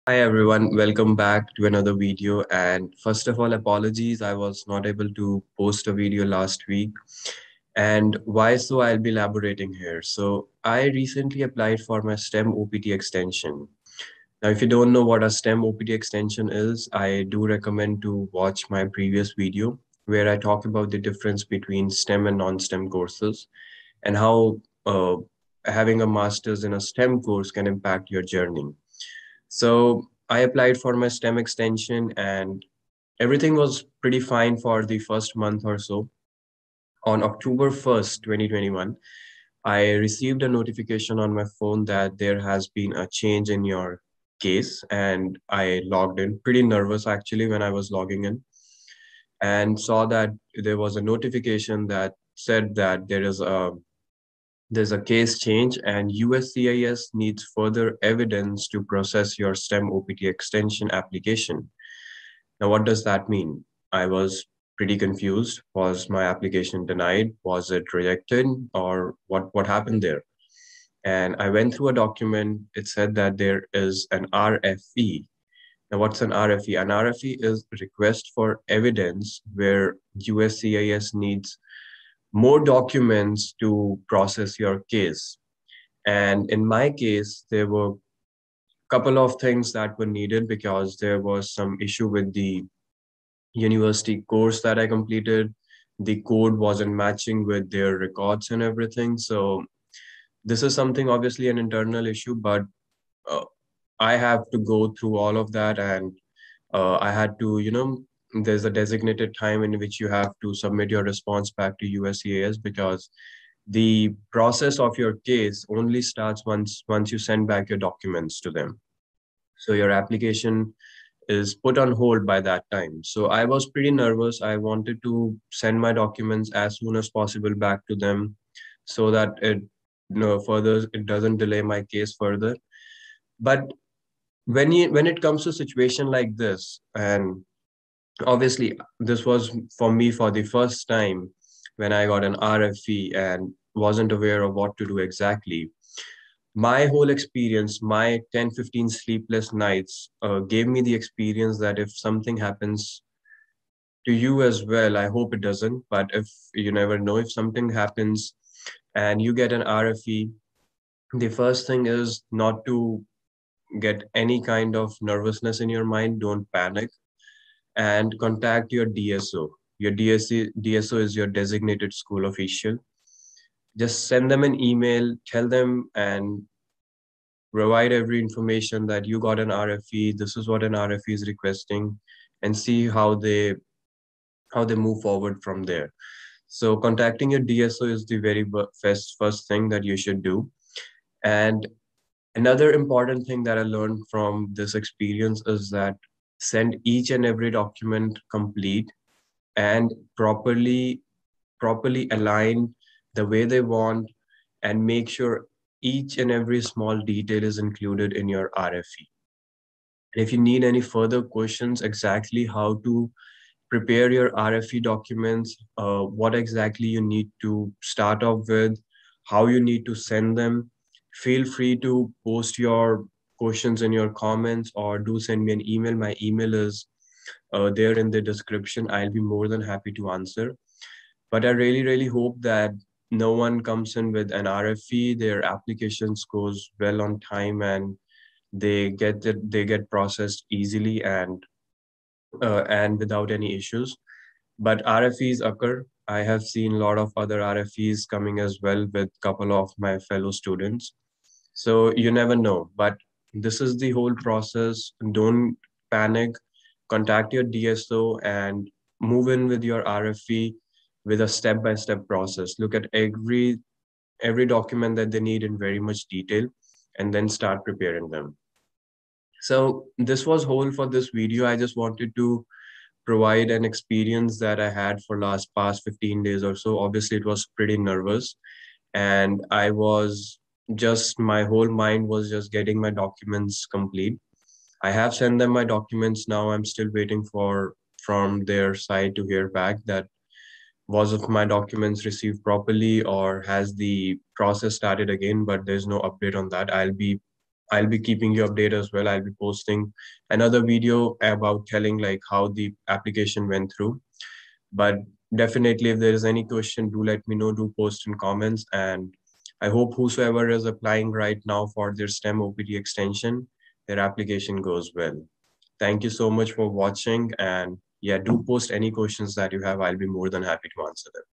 RFE and STEM OPT: How To Respond To USCIS - Part 1
Hello everyone,

In this video, I talk about my recent experience where I received an RFE from USCIS regarding my STEM OPT Application. Must watch if you are in the same situation and don't know how to move forward with your application.